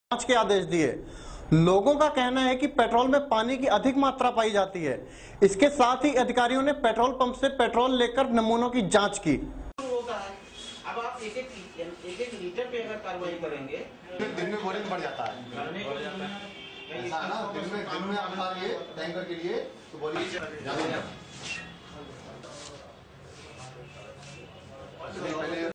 KV NEWS INDIA
मुरादाबाद में पेट्रोलपंपों की हुई चैकिंग लेकिन कोई कार्यवाही नहीं